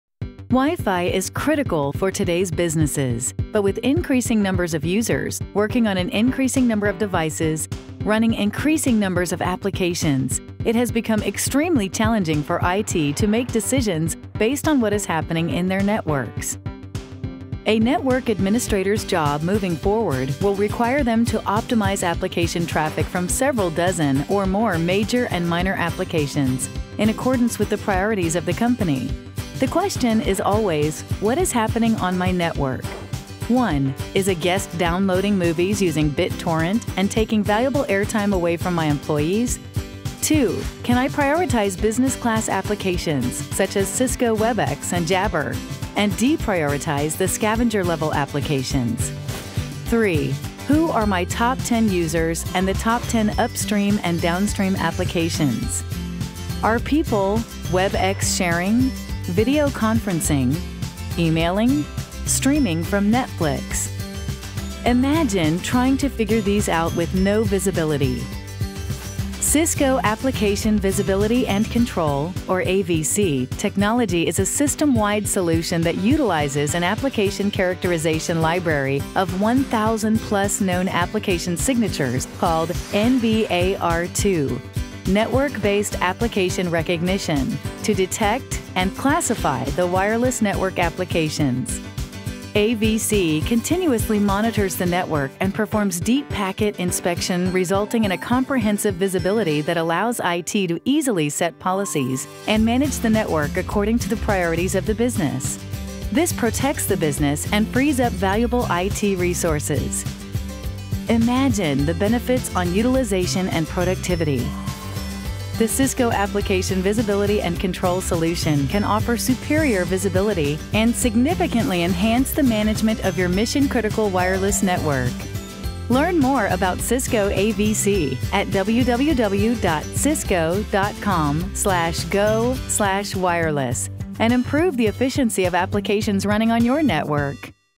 Así funciona Cisco Application Visibility and Control - AVC
Cisco Application Visibility and Control es una solución para todo el sistema que utiliza una biblioteca de caracterización de aplicaciones de más de 1500 firmas conocidas.